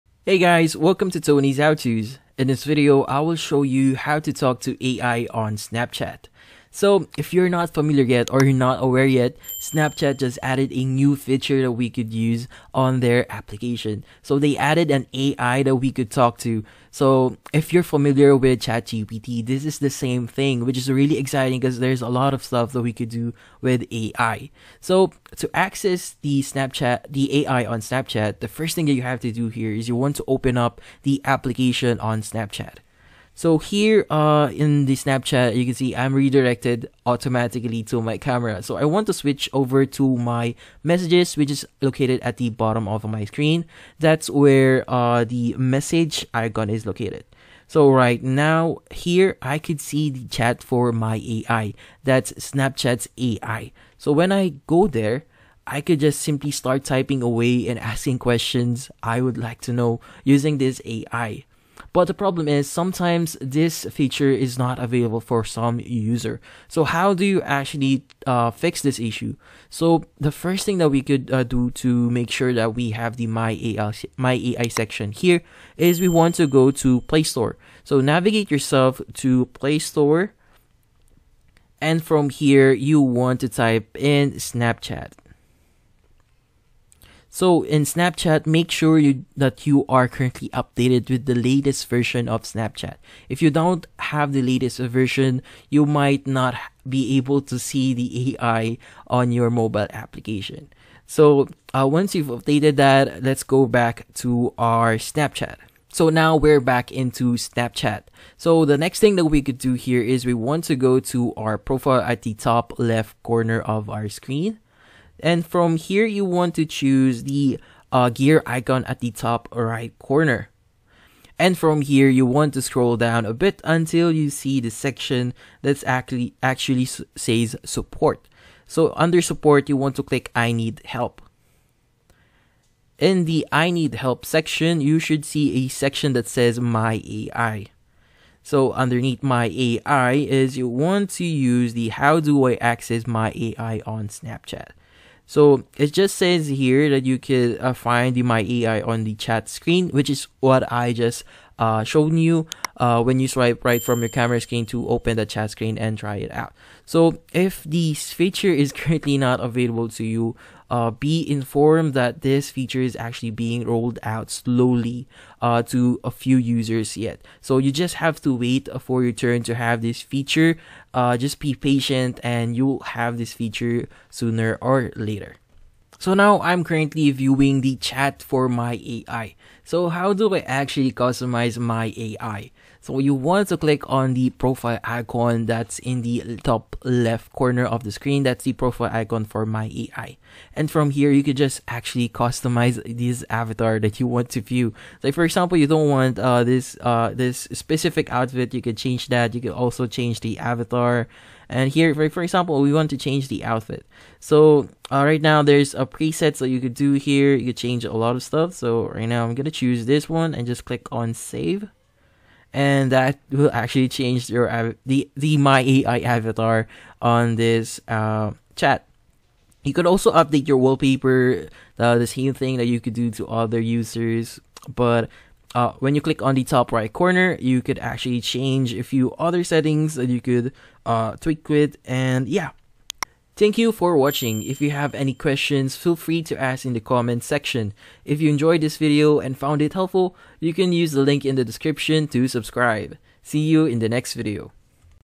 How to Talk to AI on Snapchat (2025)
In this video I show you how to talk to AI on Snapchat in 2025.
Do you want to talk to AI in Snapchat in 2025? Yes? Great!

I will show you the quick and easy tutorial how to talk to Snapchat AI.

This is a step by step instruction on how to talk to AI chatbot on Snapchat. This is valid for iPhone models 8 or higher with iOS 14 and higher. This is valid for Samsung, Google Pixel, Xiaomi and other phones running Android version 11, 12, 13, 14.